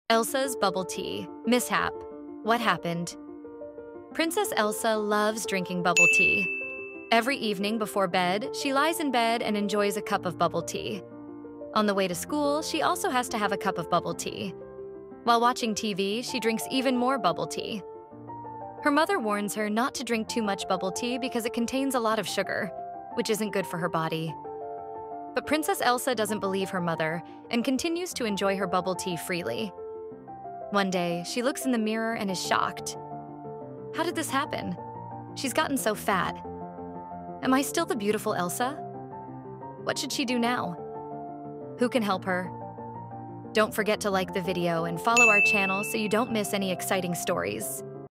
🥤 How Did Elsa’s Bubble Tea Obsession Lead to This? Comment Below! 🧐 😲
Princess Elsa loves bubble tea 🧋, but what happens when she drinks a little too much? From bedtime sips to TV binges, her bubble tea obsession might have gone too far! Watch as Elsa discovers a shocking change and needs your help. How would you handle her situation?